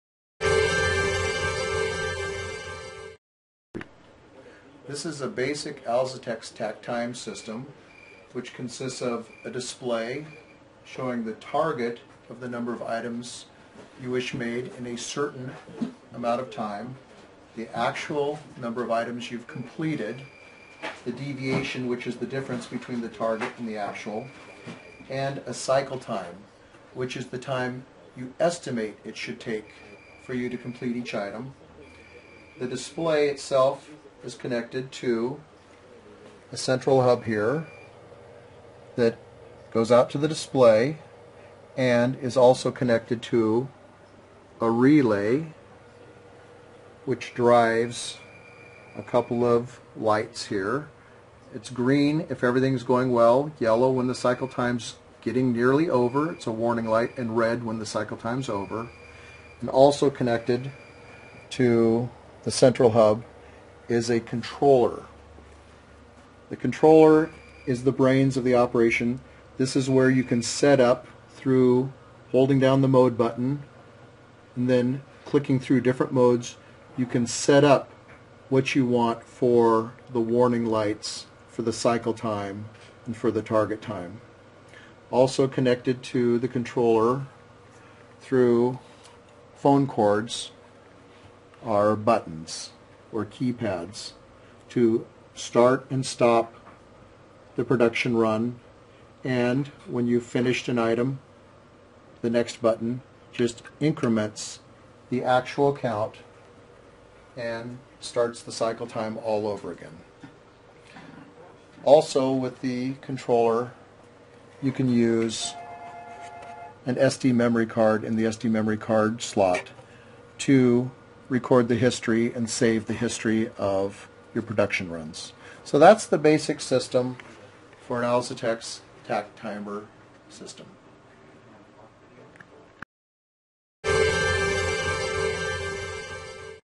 Alzatex Takt Timers
Understanding Takt Time by Alzatex Inc.
Many have a hard time understanding what takt time is when it comes to Lean Manufacturing. In order to understand takt time, let's imagine that you are a young child, and you have a paper route that you do before you go to school. The papers get delivered an hour before you have to go for your classes. So it would appear that you have sixty minutes to deliver all of your news papers. However, if you think about it, do you really have a full sixty minutes? Perhaps not.

We must remember you have to get your bike ready and load your papers on your bike. For this example let us consider you can roll your papers while you're pedaling your bicycle. You would also need to park your bike and put away your bag when you finish your route. So that hour really turns into more like fifty minutes.

Besides the time available, what else do you need to know to calculate the takt time for a newspaper delivery? Something else to consider is how many papers you actually have to deliver? Let's say you have twenty five houses on your route. You now have enough information for your takt time calculations.

The amount of time you have to deliver each paper is your takt time and in this case it is two minutes.
How long does it take to deliver each paper? (This would be your cycle time.) If it takes you less than two minutes, on average, you are able to meet your demand. If it takes longer, you are not going to get to school on time.

When there is a process that is extremely stable, like on an assembly line, it is easy to pace to the takt time. The work advances at a set pace, so the product rolls off the end in very predictable increments. This isn't the situation with a newspaper route.

Why? Well, because there is a lot of variation. There are many variables like, the weather, the distance between houses. Some people like the paper placed neatly on the porch. Others don't mind having it placed somewhere in the vicinity of the door. Some driveways are long. Some houses are close to the street. Some of your customer's dogs are not so friendly, and so on.